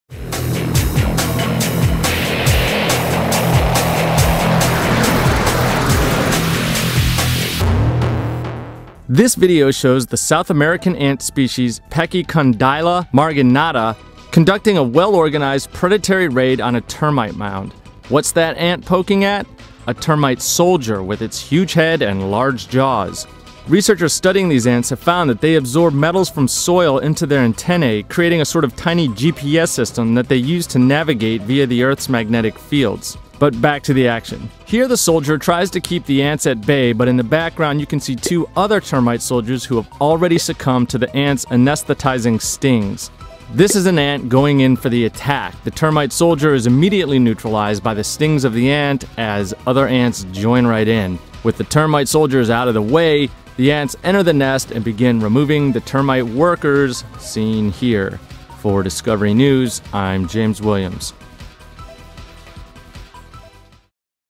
GPS Ants Raid Termite Stronghold
The same ants that use dirt particles as natural GPS units are caught on video raiding a termite mound. James Williams reports.